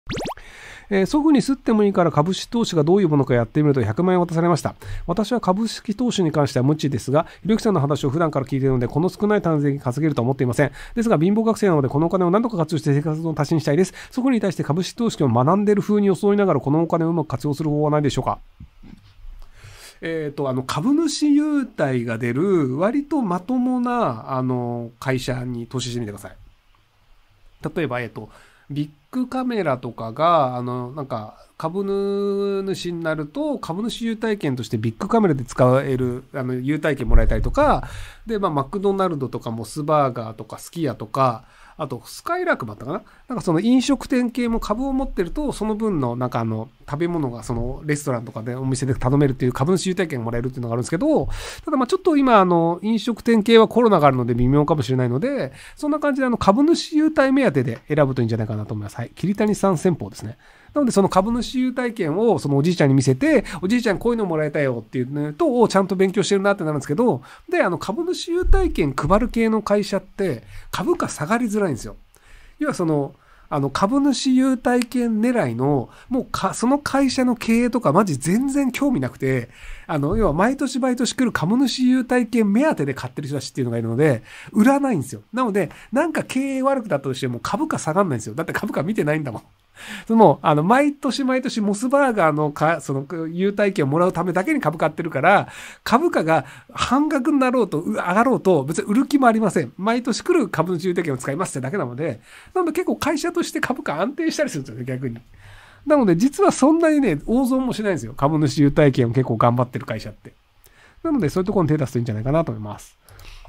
【ひろゆき】※オススメの株※ もしこれから株を始めるならまずはこの会社の株を買ってみよう【切り抜き/論破】
１％の努力 （西村ひろゆき 著）